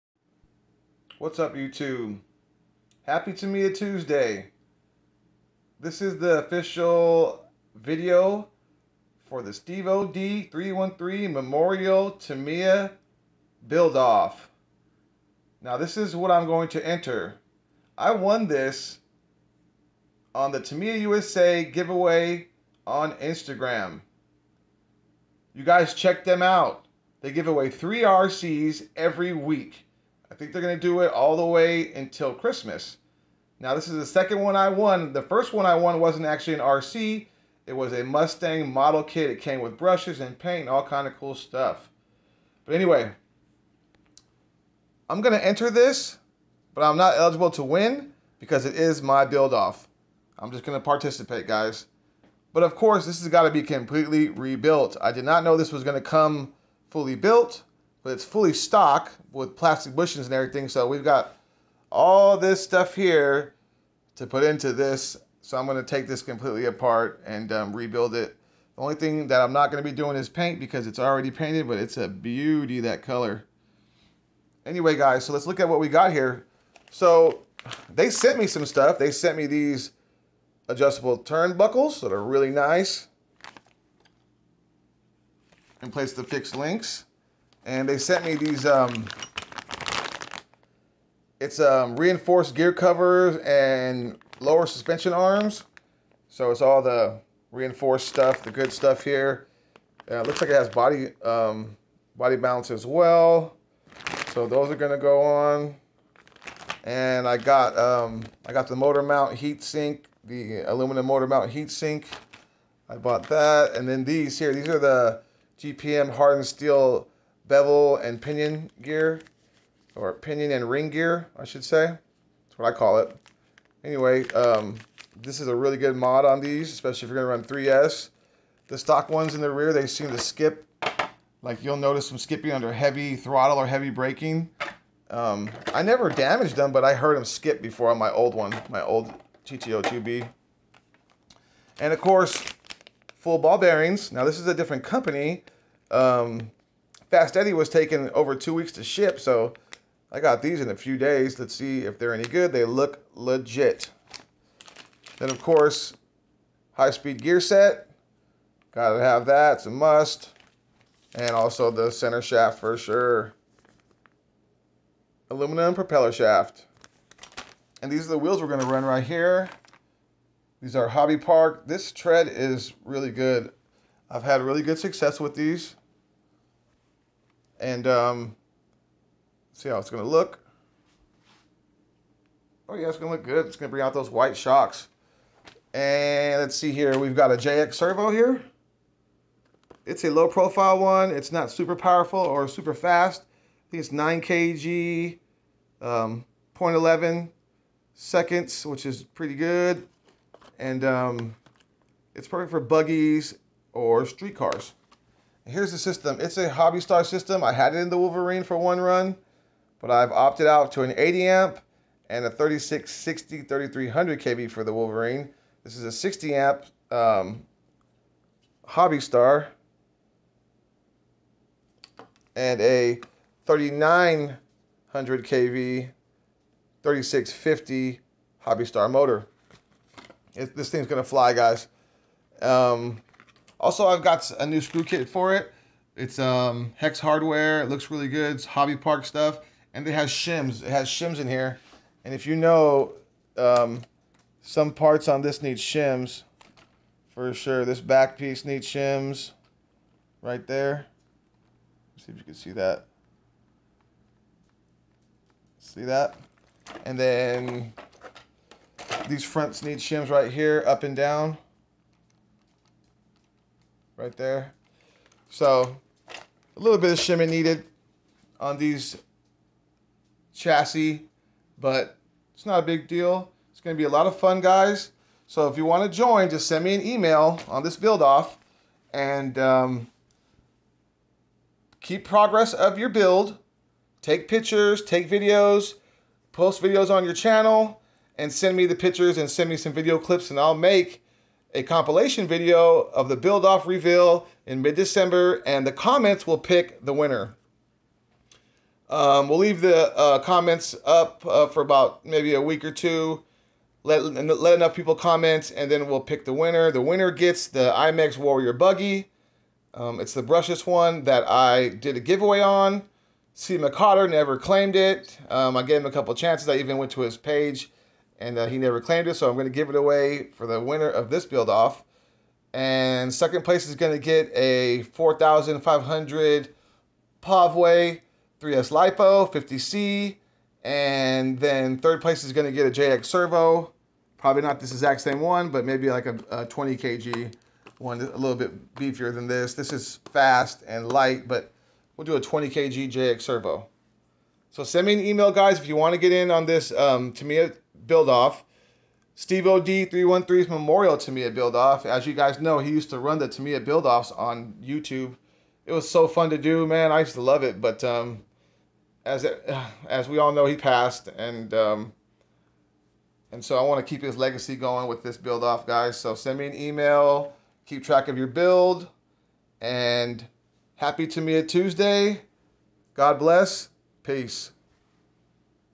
Tamiya Tuesday - Stevo D 313 Memorial Tamiya Build Off
Let's do this for Stevo guys! He was a great guy in the RC community so let's keep his memory alive!